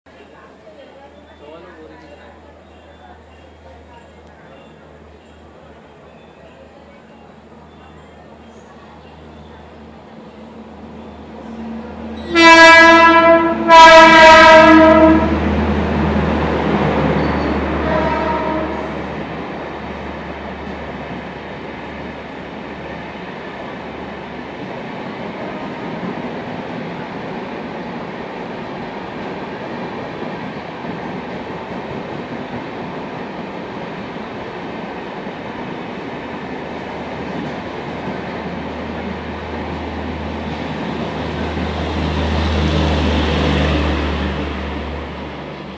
MY MAIDEN CAPTURE OF SHATABDI WITH BRC WAP 5 AT VIRAR
this is my first ever capture of Mumbai Ahmedabad shatabdi in LHB rake and that to with BRC WAP 5 30050 even i didn't captured a video of shatabdi with WCAM2P

Ahmedabad-Mumbai Central Shatabdi/12010 with BRC WAP 5 #30050
at Virar station while i was onboard Dahanu Mumbai Passenger/59462  toward my final destination Borivali i was expecting overtake at Virar and that's happen shatabdi slowly passed from station with roaring EOG

Date = 14/7/2012
Location = Virar
Time 8:18
Camera = Sony Ericsson Vivaz
Taken by = Akshay Patil